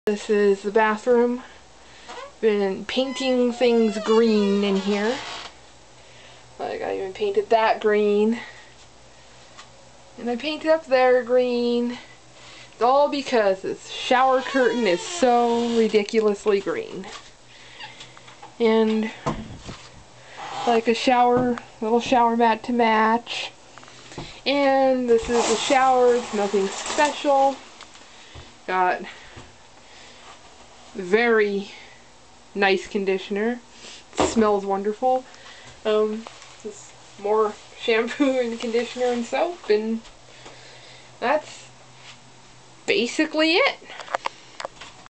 Small tour of my small bathroom
Just more video footage for my family that lives far away.